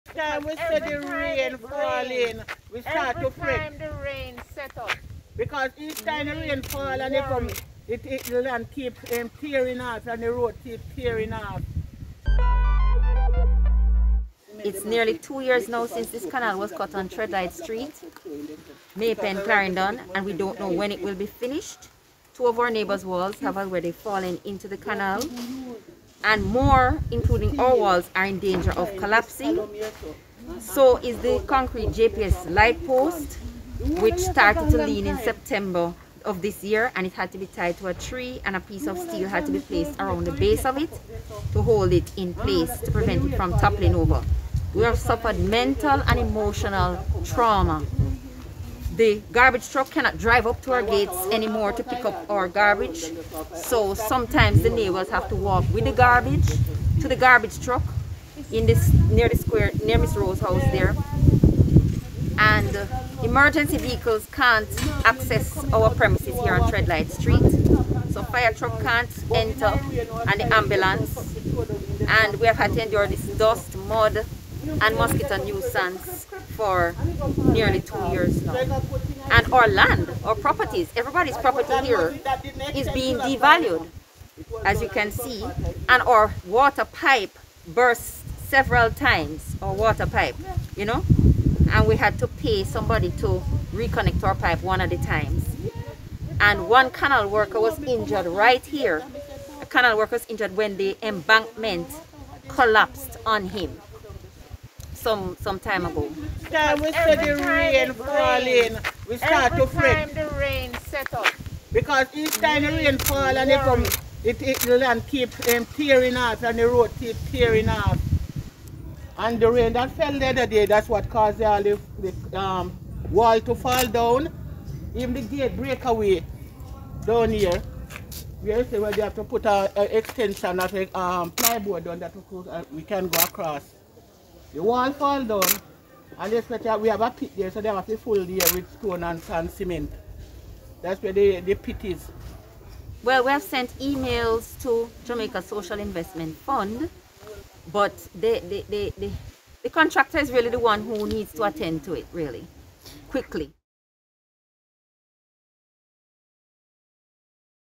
Treadlight residents longing for relief
Residents of Treadlight in Central Clarendon say they are fearful and
completely stressed as they observe the rapid deterioration of their
living environs due to a canal work that is taking almost two years.
In May of this year, several residents voiced their concerns about the
“stop and start” work that was being done on the canal which started
in December 2020.